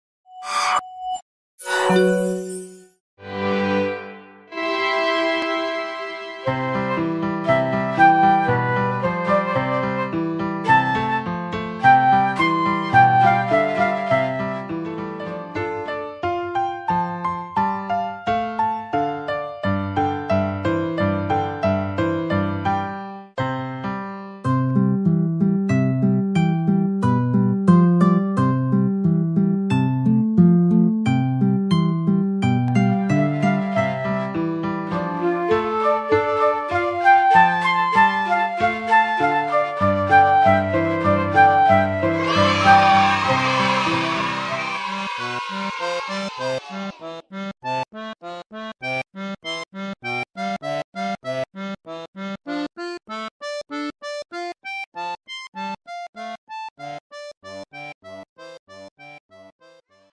Tap The Melody mobile app trailer (iPhone & iPad)
Wonderful music application for kids! Discover the world of music in an easy and engage way.